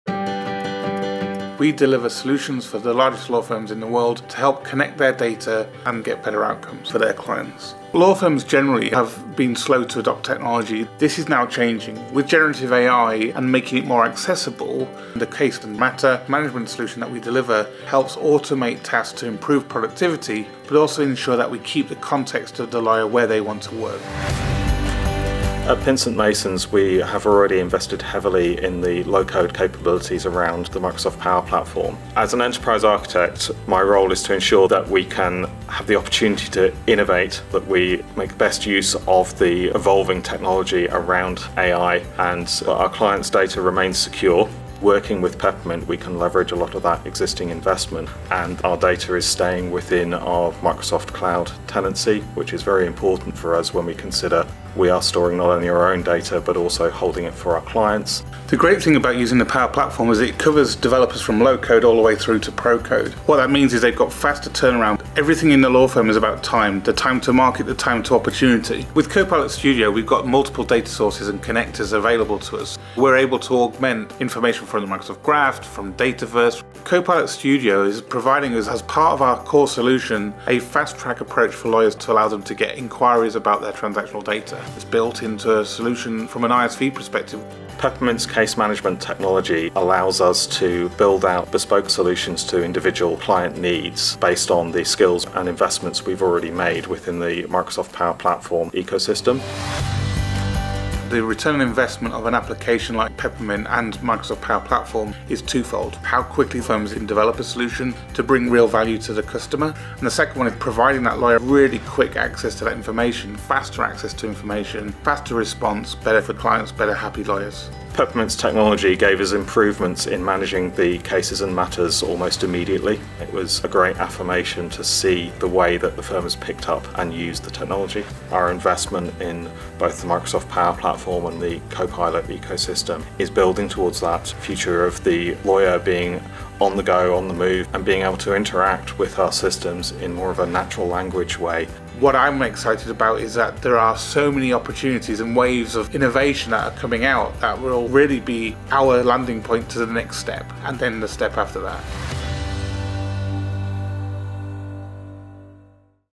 Peppermint Technology delivers solutions for law firms using Microsoft Power Platform and Copilot
Peppermint Technology delivers solutions for the largest law firms in the world to help connect their data and get better outcomes for their clients. Pinsent Masons has invested heavily in the low-code capabilities around the Microsoft Power Platform, and working with Peppermint, they can leverage that existing investment.

Peppermint's case management technology allows them to build out bespoke solutions to individual client needs based on the skills and investments they've already made within the Microsoft Power Platform ecosystem.

Find out more with Nirav Shah's latest blog post from Microsoft Build 2024: